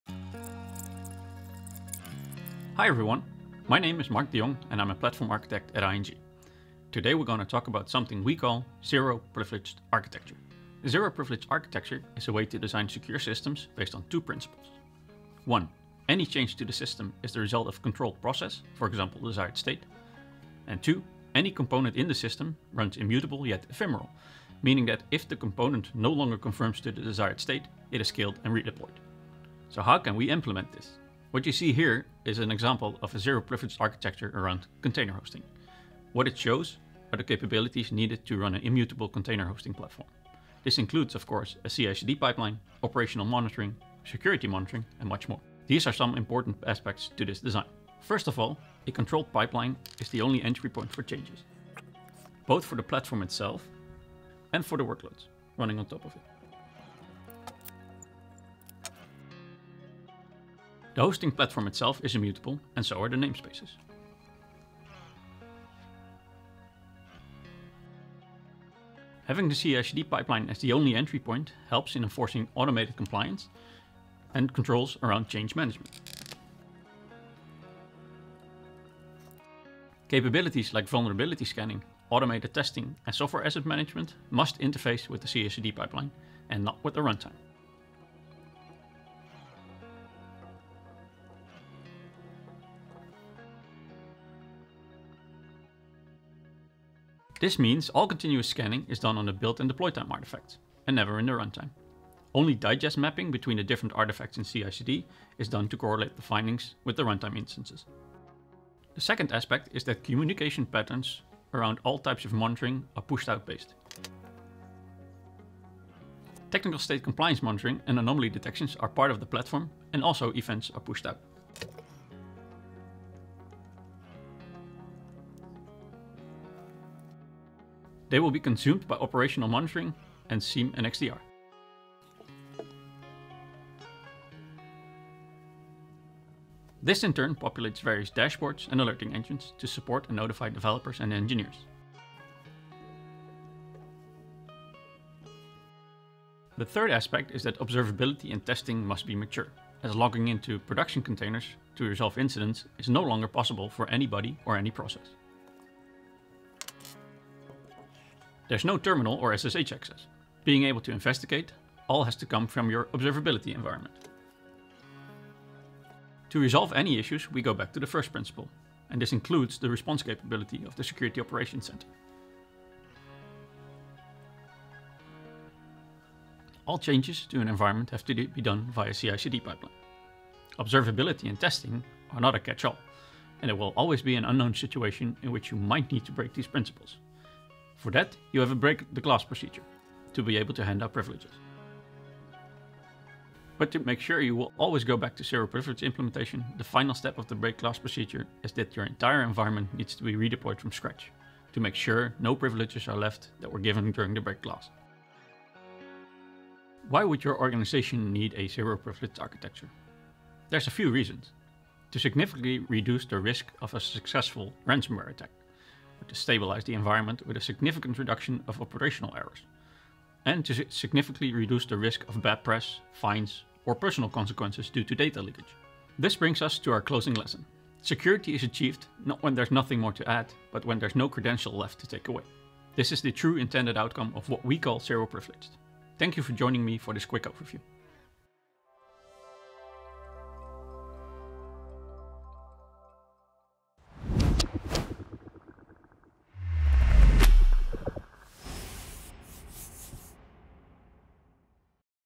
A Zero Privilege Architecture Implementation
In this lightboard video, Mark de Jong of ING, an OpenShift Commons community member, explains how to implement Zero Privilege Architecture around container hosting.